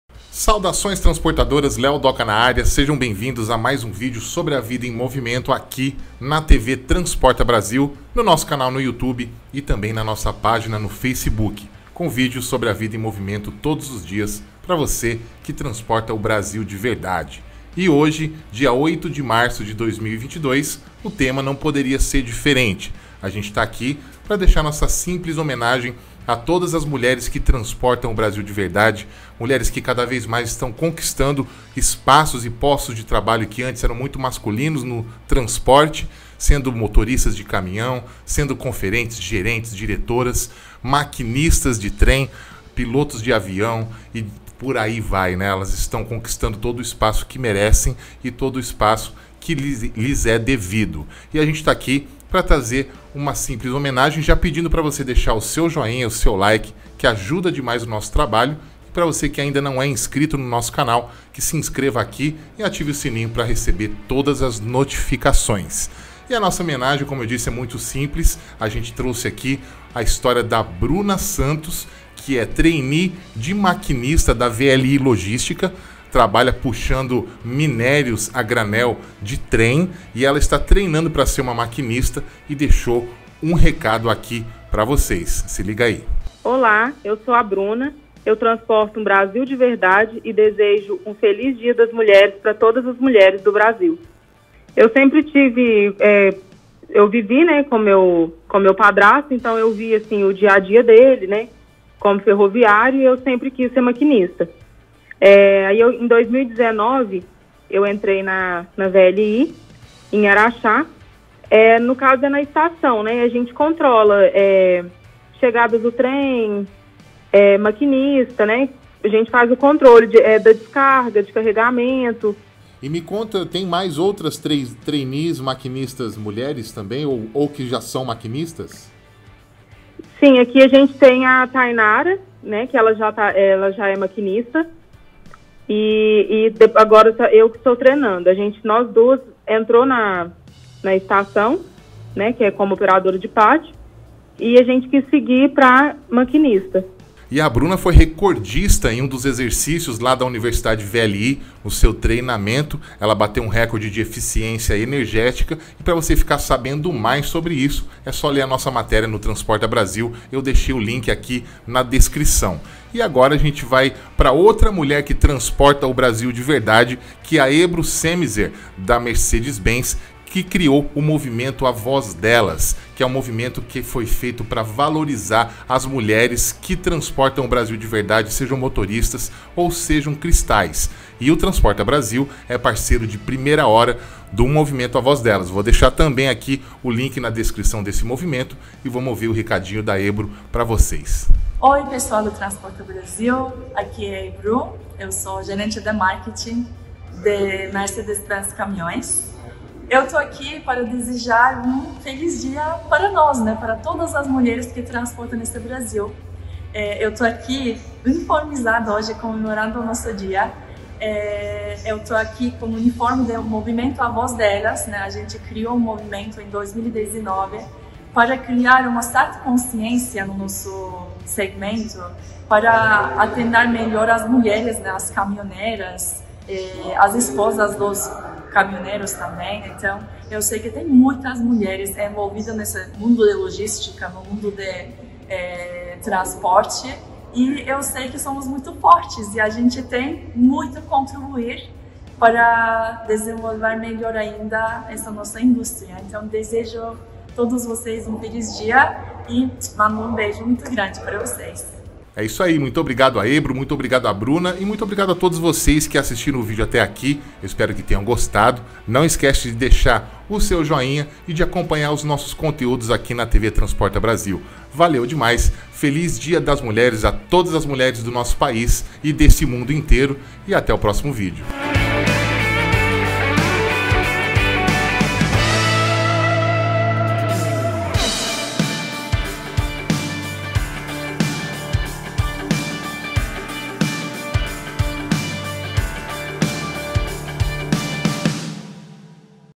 HOMENAGEM ÀS MULHERES QUE TRANSPORTAM O BRASIL
Neste 08 de março, deixamos aqui registrada nossa simples homenagem a todas as mulheres que transportam este país continental.